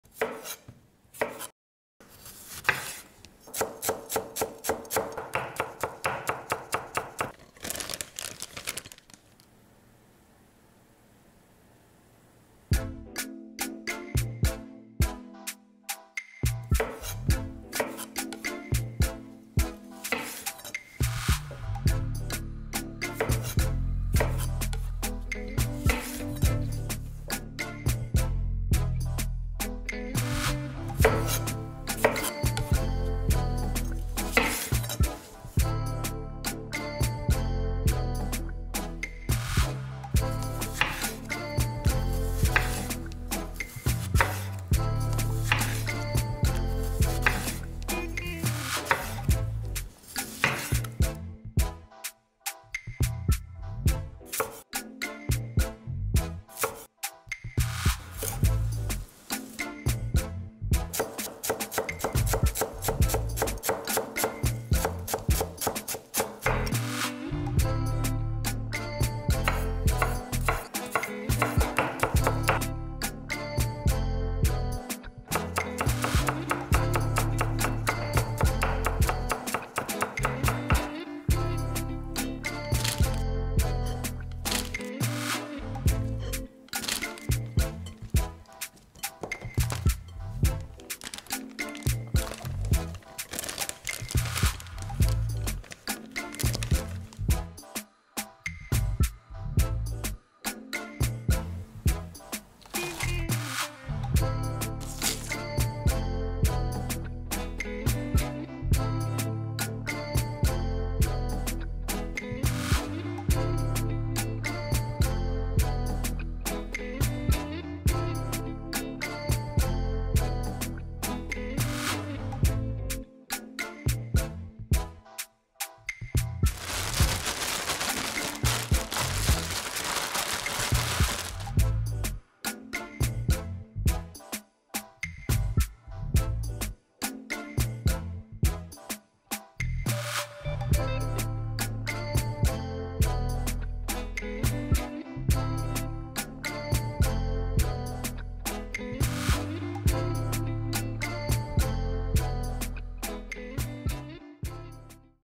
Super snack! Quick pickled cucumbers with tomatoes!
Cucumbers-700 gr.
Tomatoes-500 gr.
Garlic - 3 cloves.
Dill.
Sugar-1 tsp
Hot chili-0.5 tsp
Black pepper - 0.5 tsp
Salt-1.5 tsp